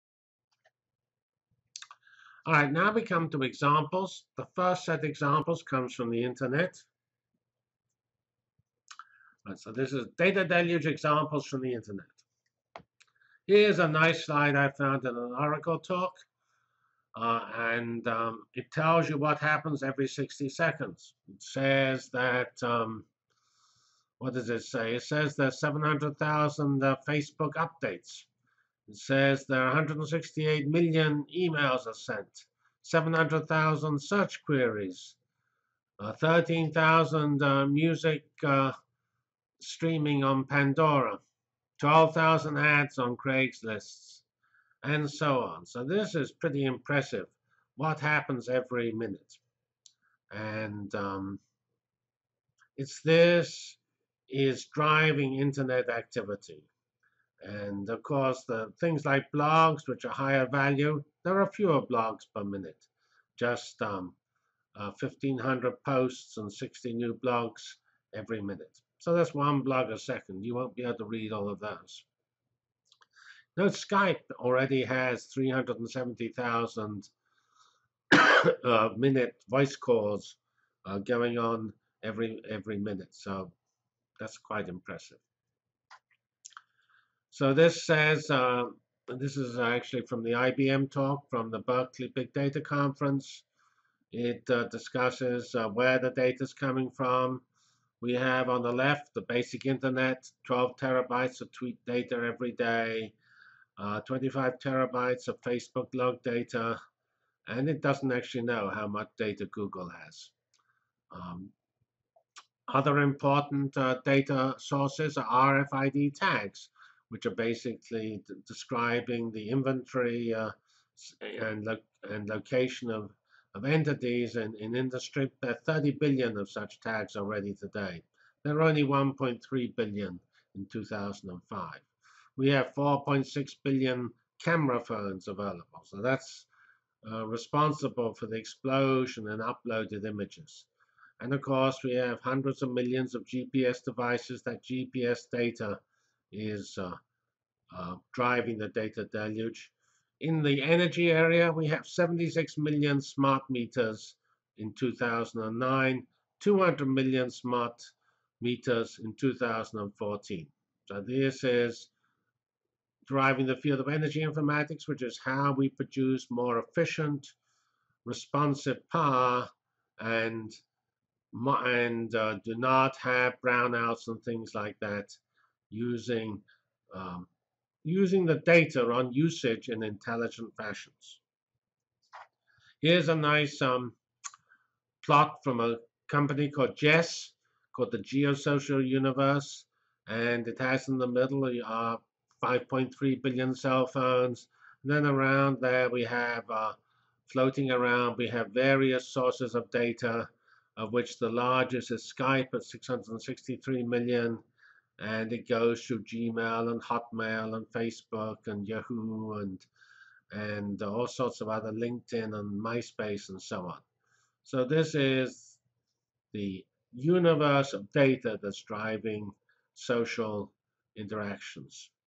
X-Informatics Unit 3: Data Deluge 5: Internet
and download course material, see information on badges and more. It's all free and only takes you a few seconds.

Lesson Overview:
Clouds and Big Data are transformational on a 2-5 year time scale. Already Amazon AWS is a lucrative business with almost a $4B revenue. Geoffrey describes the nature of cloud centers with economies of scale and gives examples of importance of virtualization in server consolidation. Then key characteristics of clouds are reviewed with expected high growth in Infrastructure, Platform and Software as a Service.